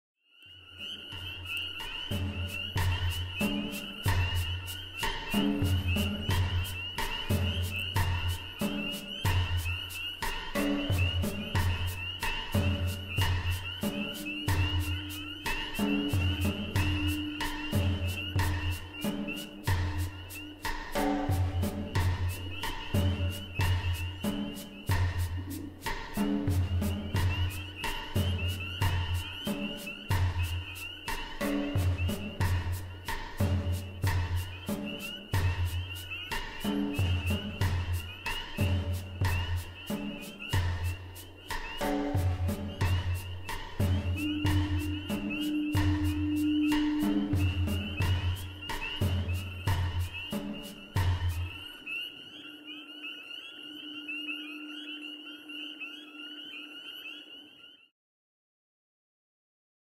Breton Bone
A whale bone in Cape Breton NS points back to the sea, past the frogs and into the fog horn. The whale bone was harvested from a local fisherman. He cut it out from a horribly rotting corpse, and brought it back to land via dory.